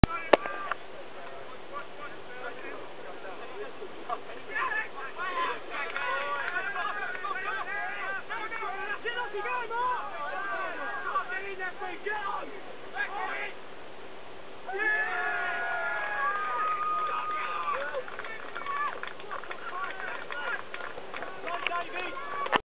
First goal of the season!
Christ Church Beckenham's first goal of the season, scored by Mark Fletcher against West Croydon Baptist Church. With thanks to WCBC for the footage